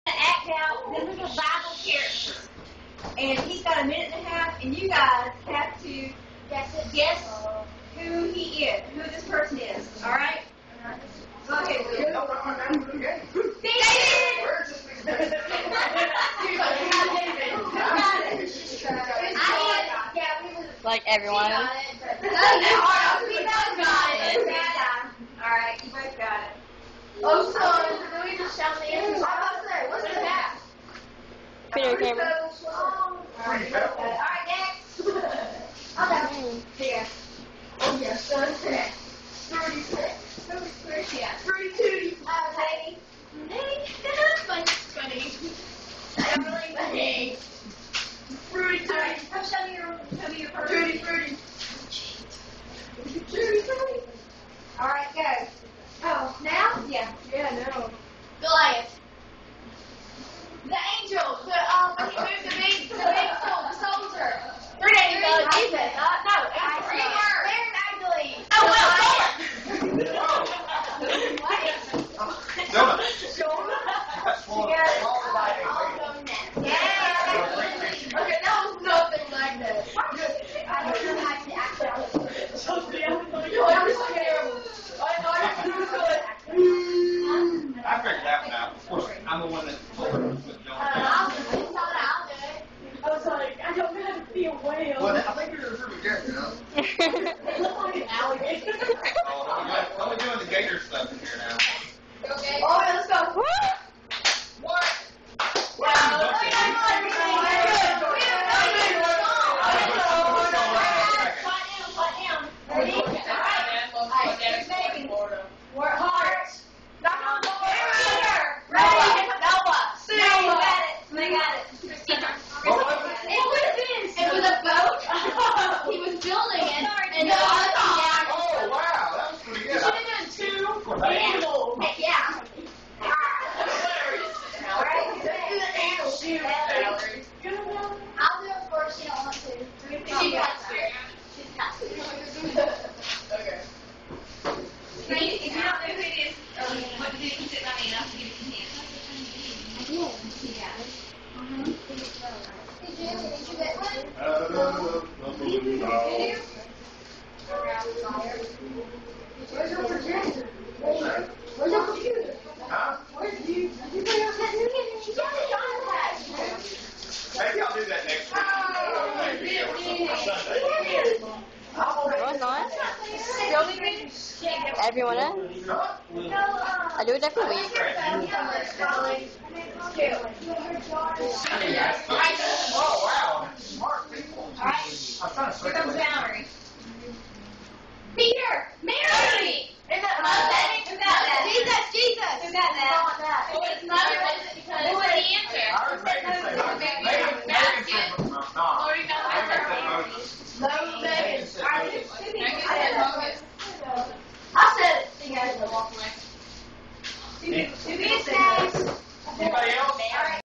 teen church 5 part 1
another fun time in teen church playing bible charades.